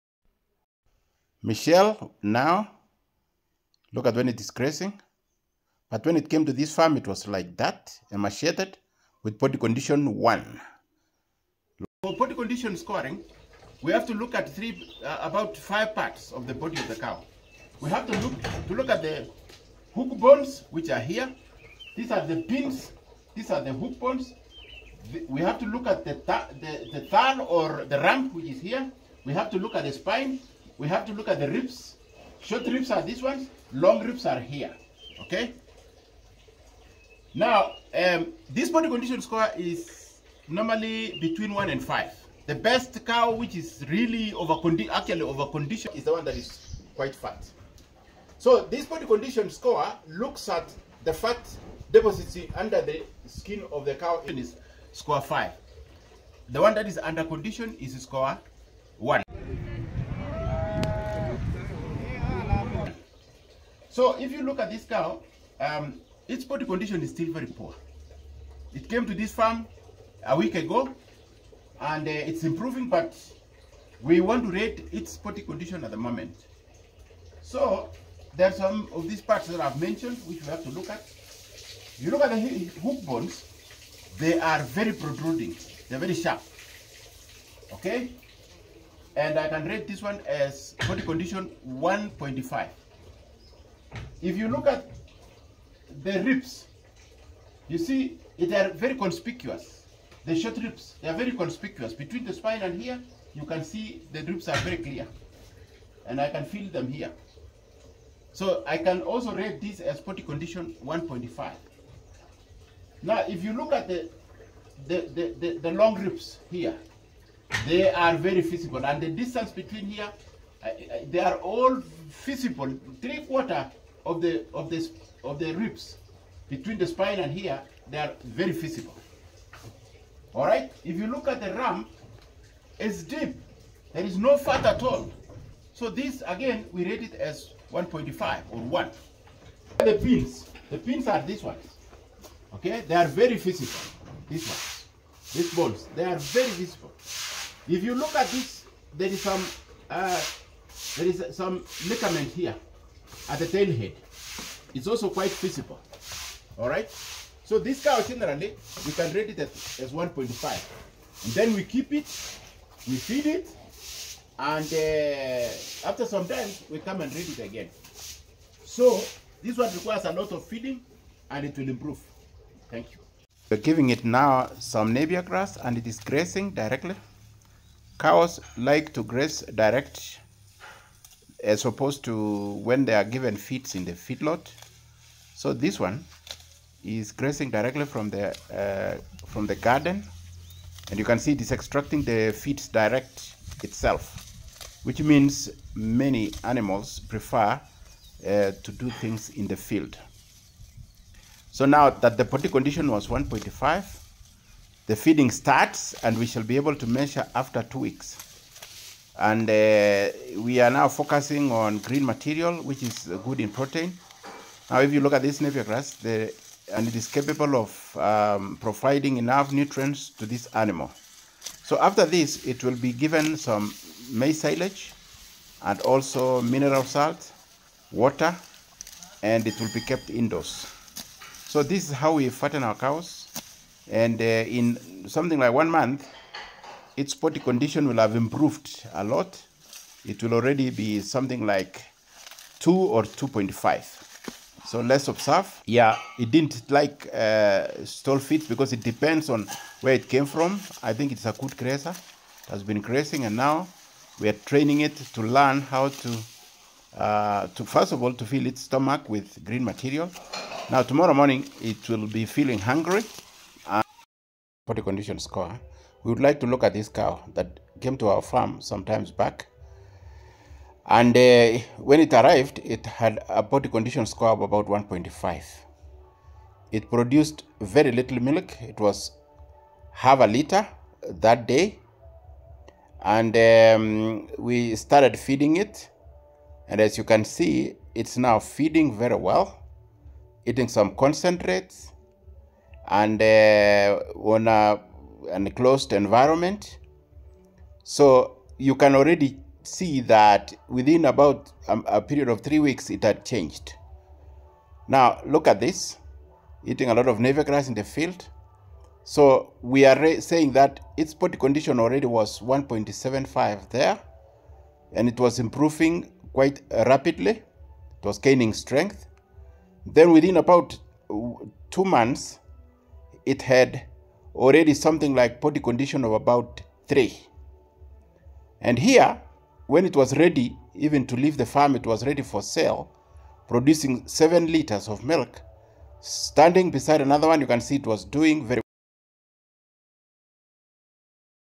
Improving the dairy cows performance by using body condition score (BCS) tool.
Did you know that dairy  cows performance depends largely  ( upto 70%) on feeding .

We demonstrate this at Seb farm. We buy poorly conditioned cows and feed with quality feeds. Normally we start with BCS between 1 to 2 (see the video) and improve it to upto BCS 5 within 3 months. To ensure this happens we regularly do the scoring.

The cows shown on the  video s  have been improved at Seb farm.